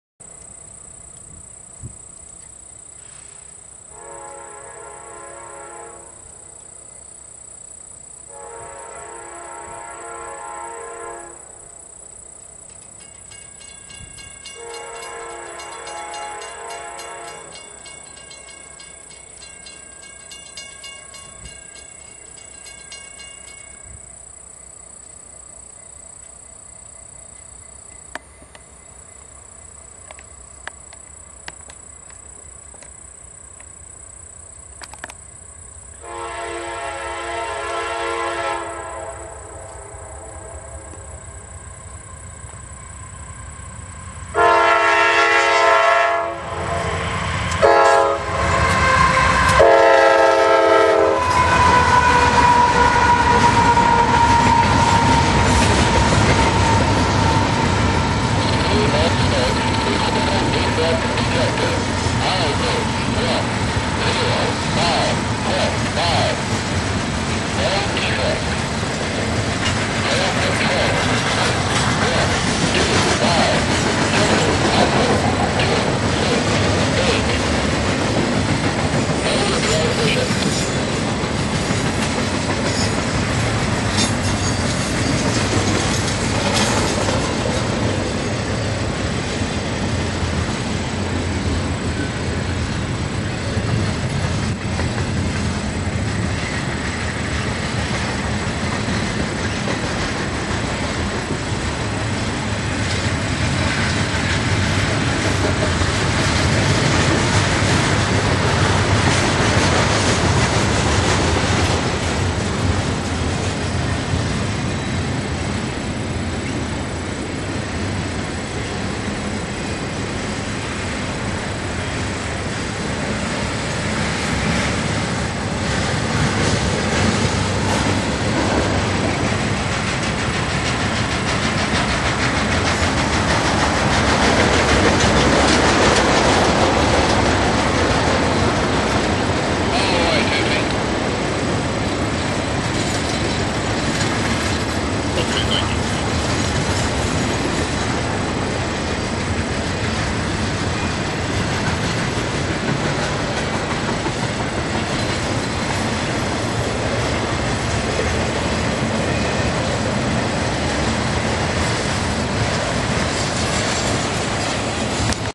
CSX 658 Leads CSX Q127-11 at North Tunnel Hill
10/12/2008
4:08 P.M. ET
CSX W&A Sub
North Tunnel Hill

Tunnel Hill, GA

CSX Q127 heads south around a sweeping curve at the north end of the the siding. You can also hear the DD at milepost 109.9 going off.